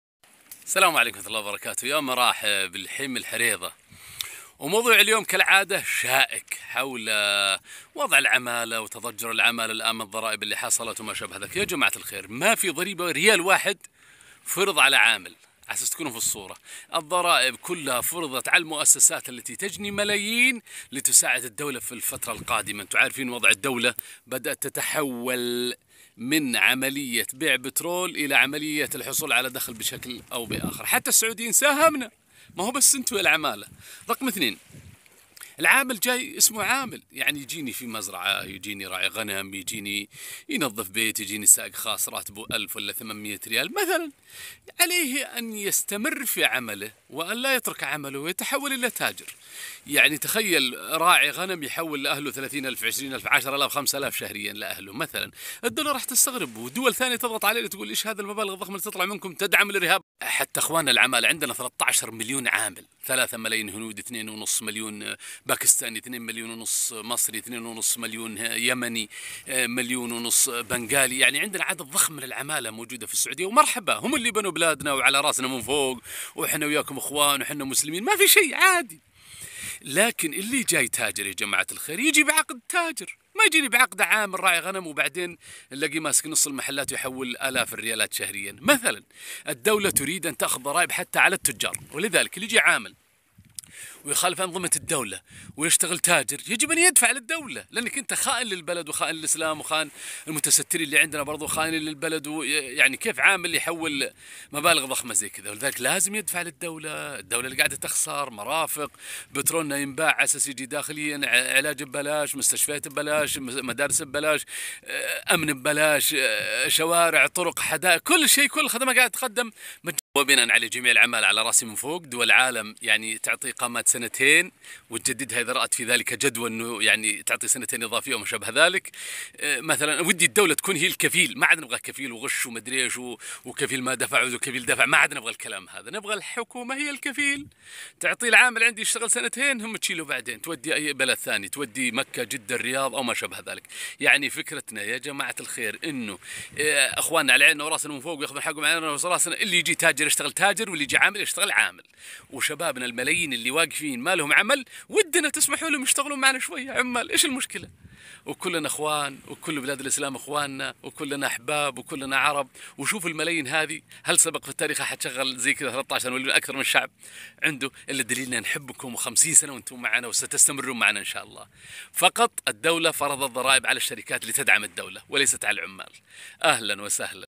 الضرائب ليست على العمال بل الشركات ومن يريد يكون تاجر لابد من فيزة مستثمر السعودية
قناة مفاتيح مع احمد بدوي الضرائب ليست على العمال بل الشركات ومن يريد يكون تاجر لابد من فيزة مستثمر السعودية